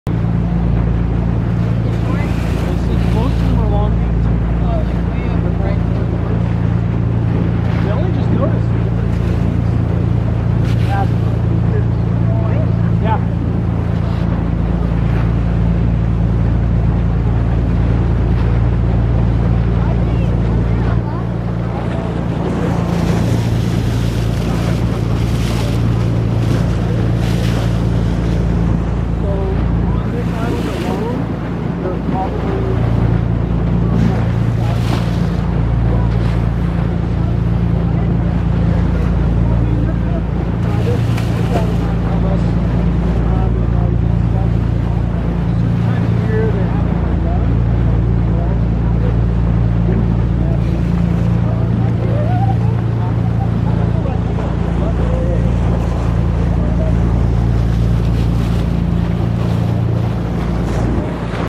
Dolphin escort across the Santa Barbara Channel
Heading back from Santa Cruz Island abord the Island Packers boat, Sept 14 2024.  Be sure to look for the mother and baby toward the end.